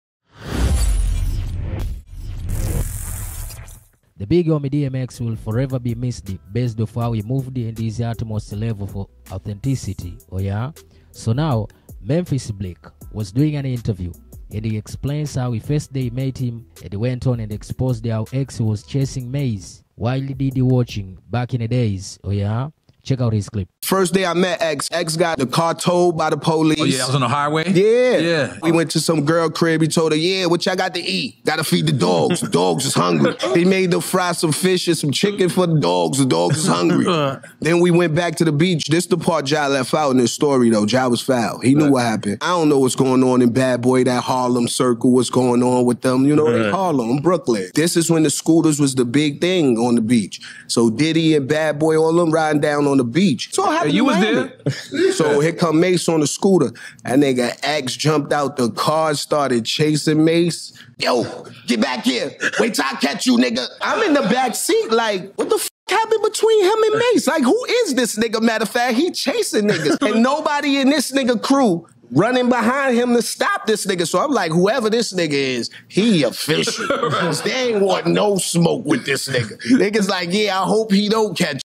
Memphis Bleek: “DMX Was Chasing Ma$e To B£AT Him. Diddy SCARED Af”
The Big homie DMX will ferever be missed based off how he moved and his utmost level of authenticity.

Oh yeah, so now, Memphis Bleek was doing an interview and he explains how he first day met him and went on exposed how X chasing Ma$e while Diddy watching.

Check out his clip.